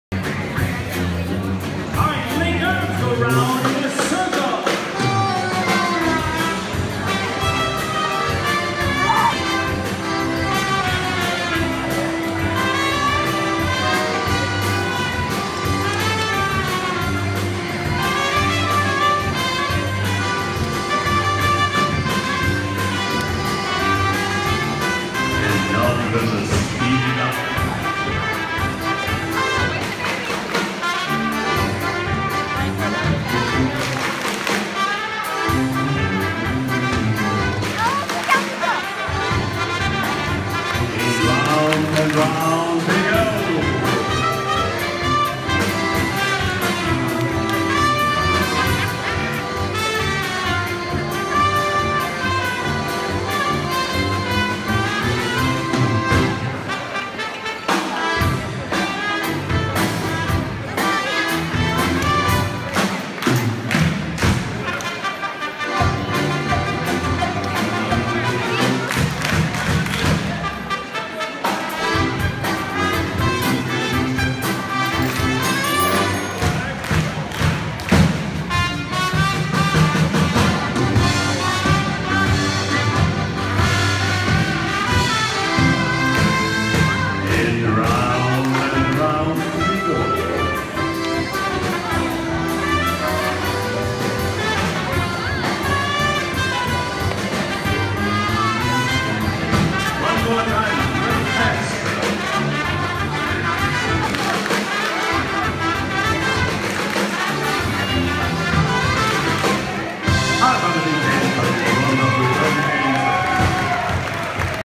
Biergarten Restaurant at Epcot/Germany
The front stage show at the Biergarten-Germany /Epcot